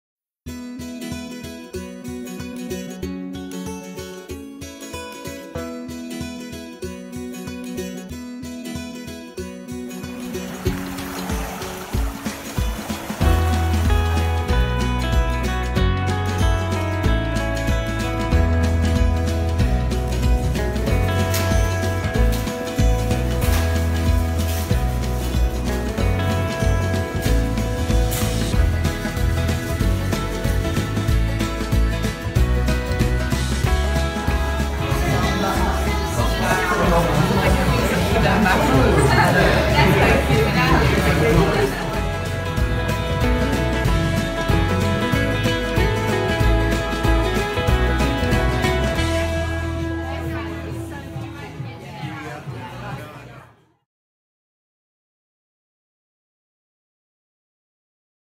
Nuestro Febrero'17 en un minuto
Despedimos un febrero maravilloso, con un tiempo envidiable y la visita de los clientes más amables y divertidos. Gracias por vuestra confianza!